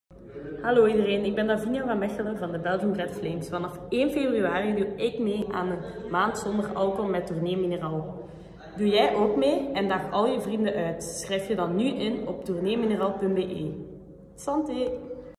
Davinia Vanmechelen - Ambassadeur Tournée Minérale 2020 - Stichting tegen Kanker en Druglijn
Tournée Minérale komt terug voor een vierde editie. Doe mee met een alcoholvrije maand februari 2020.